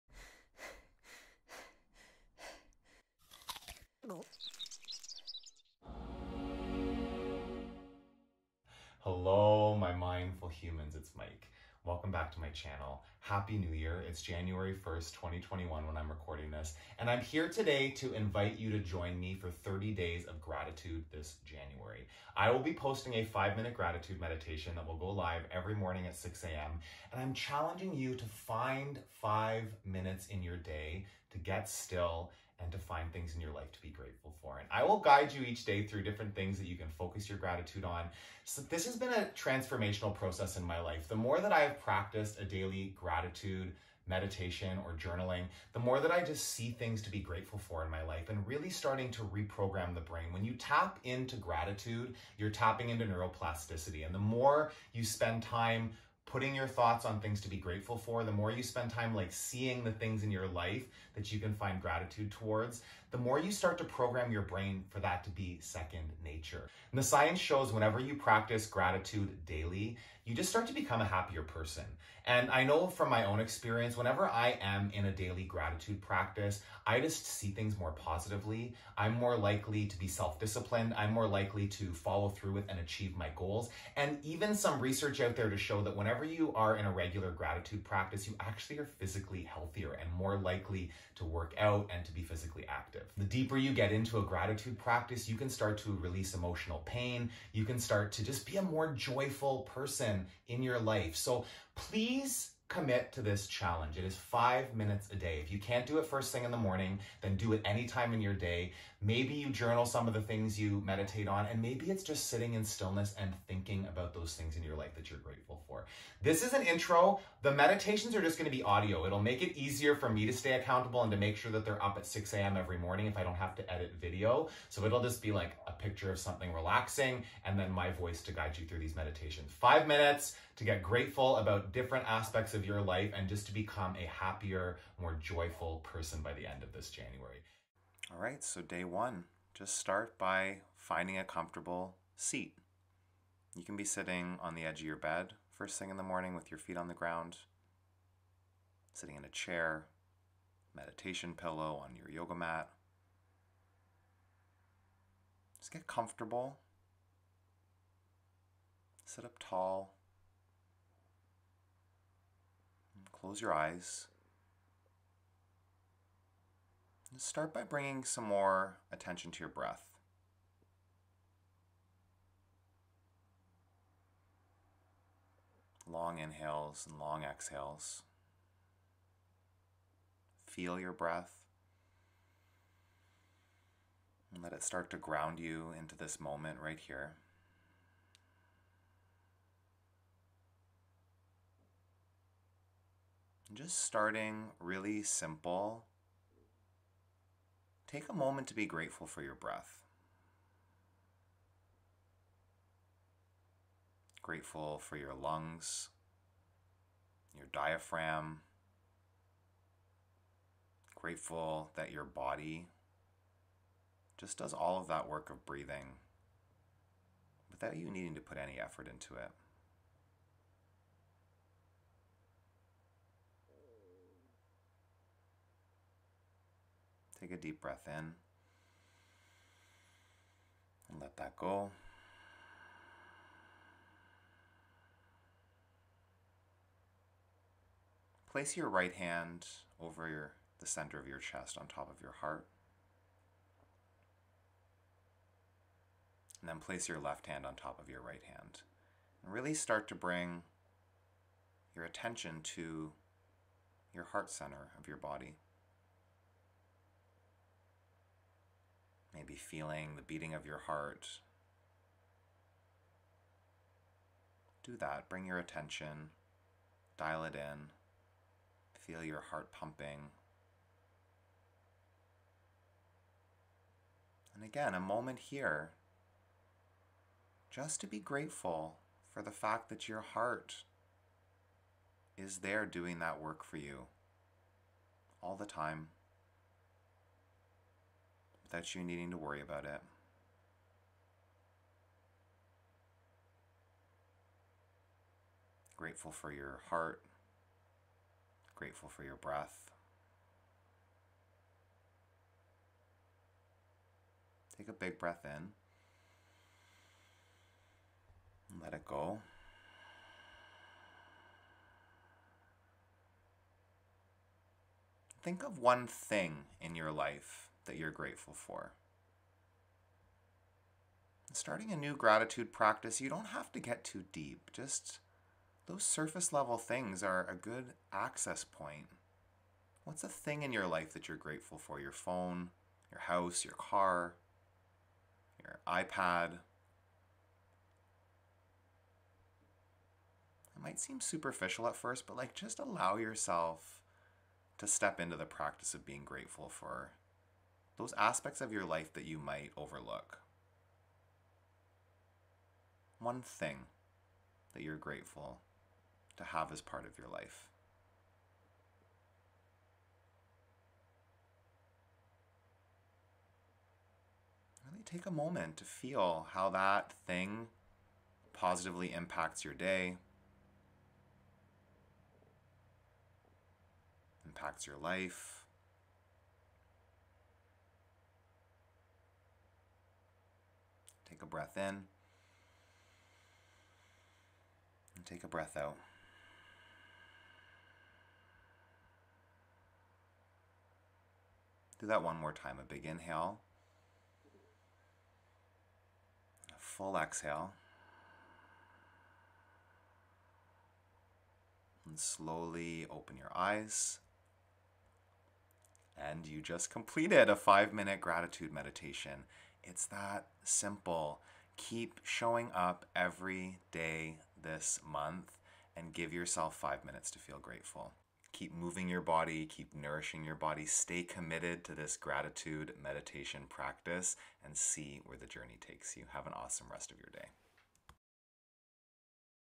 Gratitude Challenge Day 1 (Daily Guided 5-Minute Meditations ALL January!)
Join me for an entire month of Gratitude this January! Let's ring in the New Year by cultivating a daily gratitude practice together. Research shows that people who regularly practice being grateful are happier, more self disciplined and better able to achieve their goals. Gratitude has also been linked to improved physical fitness and mental wellbeing.

I commit to uploading 5-minute audio meditations EVERYDAY for the entire month. They will be there each morning by 6am EST. We will start VERY basic and each day focus on a different area to appreciate. All you need to do is click on the video, get still, listen and feel grateful. Simple right?? Commit to a new habit this year and see where this gratitude journey takes you!